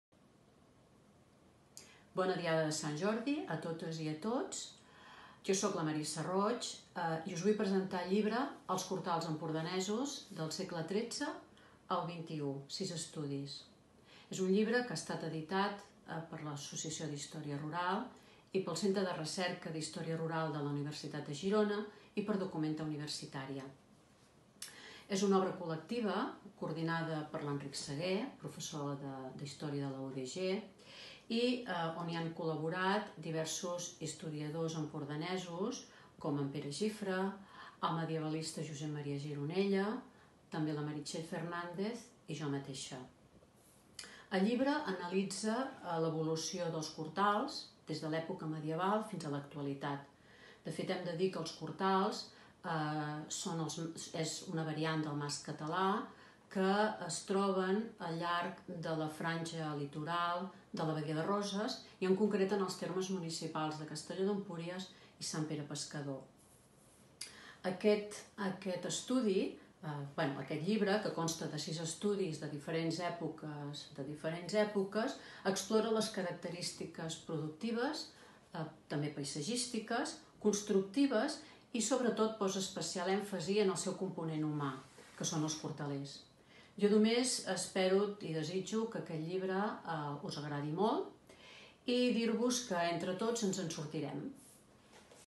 Roig, Marisa
Marisa Roig presenta el llibre "Els Cortals empordanesos del segle XIII al XXI : sis estudis" a la Revetlla virtual de Sant Jordi 2020 de la Biblioteca de Figueres.